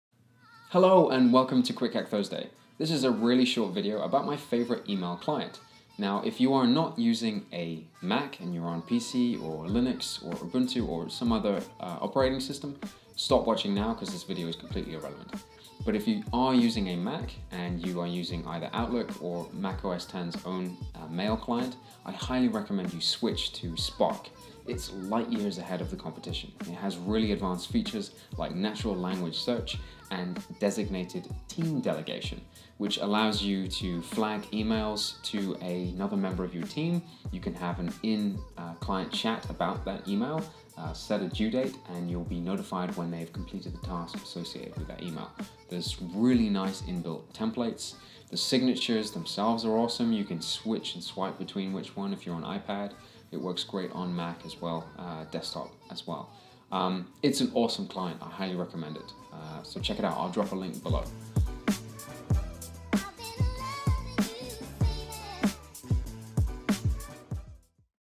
Drop outlook & Mac Mail, start using Spark
⏰If you're on Mac, you need to be using Spark for Email

In this super short Quick Hack Thursday, here's my reasons for using Spark!